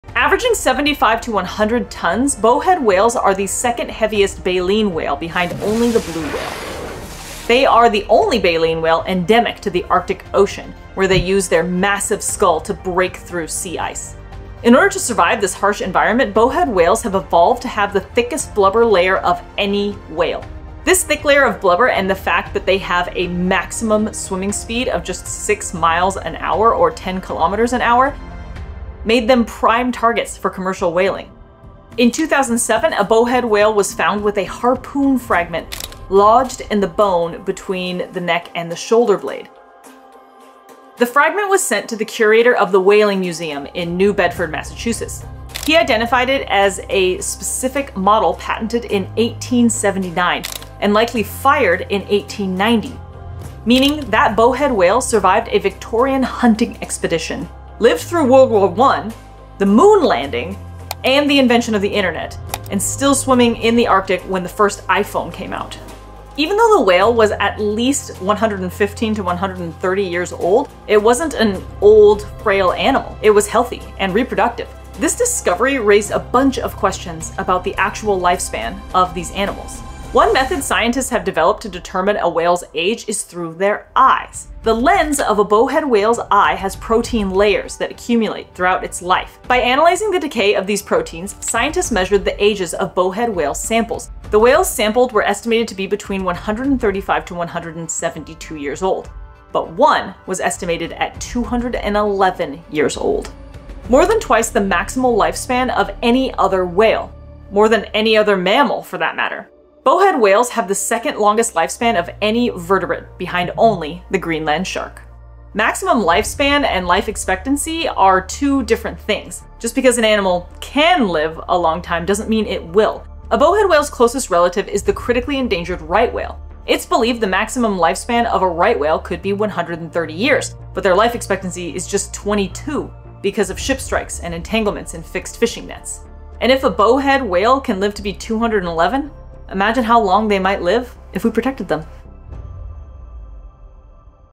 The 200-Year-Old Whale Swimming Beneath Arctic Ice
Averaging 75 to 100 tonnes, bowhead whales are the second heaviest animal behind only the blue whale. They are the only baleen whale endemic to the Arctic Ocean, where they use their massive skull to break through sea ice. In order to survive this harsh environment, bowhead whales have evolved to have the thickest blubber layer of any whale. This thick layer of blubber and the fact that they have a maximum swimming speed of just 6 miles an hour or 10 kilometers an hour… made them prime targets for commercial whaling.  Commercial whaling of bowhead whales effectively ended in the early 1900s, with whaling ceasing by 1921. By then, the population had declined to less than 3,000 individuals. Thankfully, some populations have shown considerable recovery since the end of commercial whaling, with an estimated population of 12,500 in the Western Arctic.

In 2007, a bowhead whale was found with a harpoon fragment lodged in the bone between the neck and shoulder blade. The fragment was sent to the curator of the Whaling Museum in New Bedford, Massachusetts. He identified it as a specific model patented in 1879 and likely fired in 1890. Meaning that bowhead whale survived a Victorian hunting expedition, lived through World War I, the Moon Landing, the invention of the Internet, and was still swimming in the Arctic when the first iPhone came out. Even though this whale was at least 115 to 130 years old, it wasn't an “old,” frail animal. It was healthy and reproductive. It was relatively young. This discovery raised a bunch of questions about the actual lifespan of these animals.

One method scientists have developed to determine a whale’s age is through their eyes. The lens of a bowhead whale's eye has protein layers that accumulate throughout its life, sort of like an onion (ogres are like onions). By analyzing the decay of these proteins, scientists measured the ages of bowhead whale samples taken between 1978 and 1996. These whales were estimated to be between 135 and 172 years old. But one was estimated at 211 years old. More than twice the maximal lifespan of any other whale. More than any other mammal, for that matter. Bowhead whales have the second longest lifespan of any vertebrate behind only the Greenland shark.

Maximum lifespan and life expectancy are two different things. Just because an animal CAN live a long time doesn’t mean it will. Killer whales have a maximum lifespan of about 80 to 90 years. But the average life expectancy for the southern resident killer whales is just 29 years for females and 17 years for males. That doesn’t take into account that less than 50% of calves make it to their first birthday. A bowhead whale’s closest is the critically endangered right whale. It’s believed the maximum lifespan of a right whale could be 130 years, but their life expectancy is just 22 because of ship strikes and entanglements in fixed fishing gear. And if a bowhead whale can live to be 211, imagine how long they might live if we protected them.